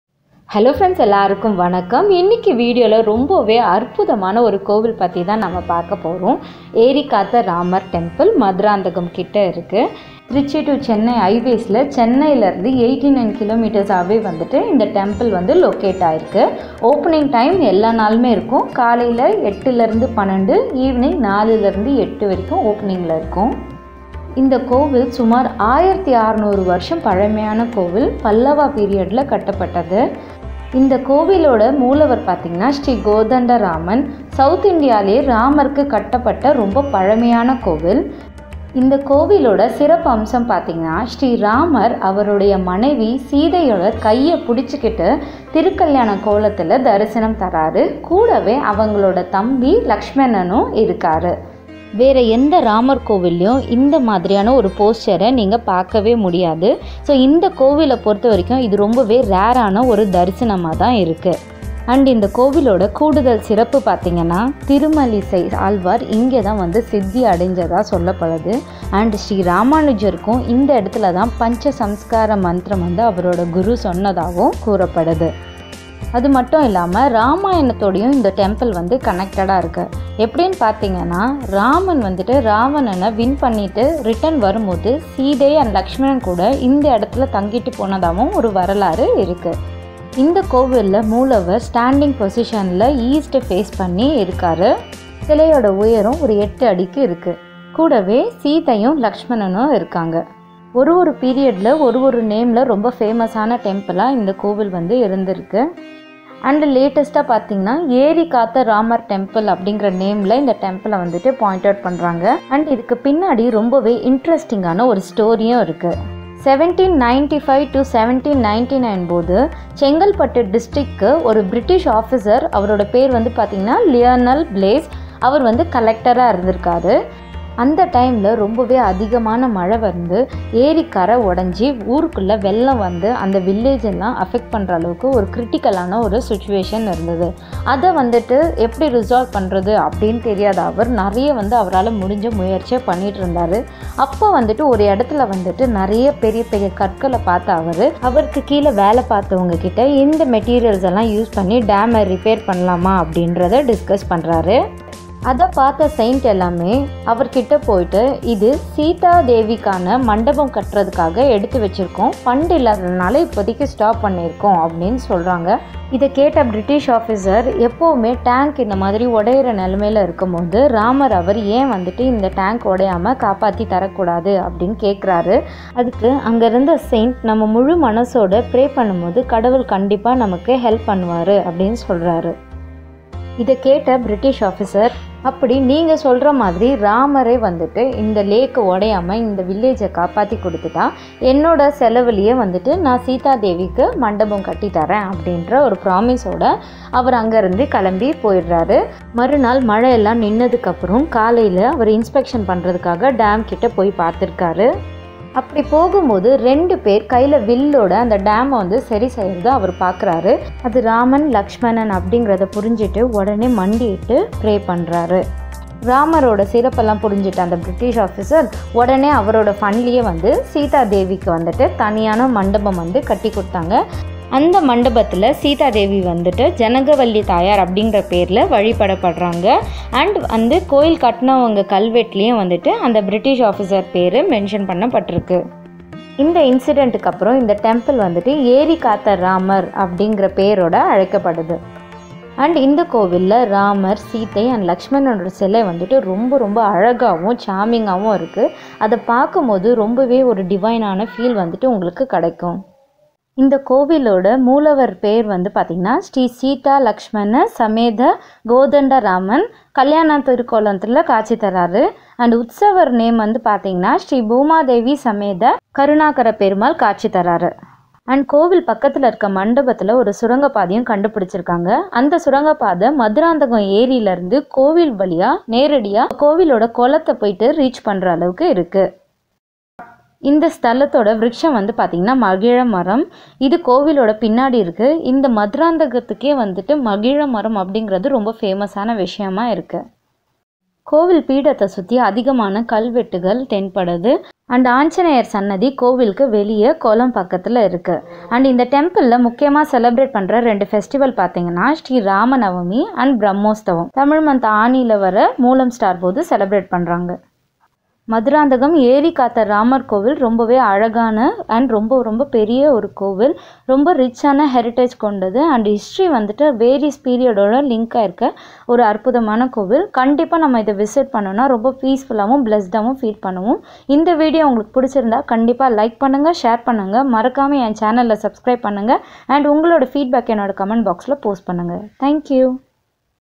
Eri Katha Ramar Temple Madurantakam / Eri Katha Ramar temple history / Place to visit near chennai
Hello friends.. In this video I showed a devotional trip to Eri-Katha Ramar Temple, oldest temple for Ramar in south India over 1600 years old... Hope you like it...

Address - Maduranthakam, Tamil Nadu 603306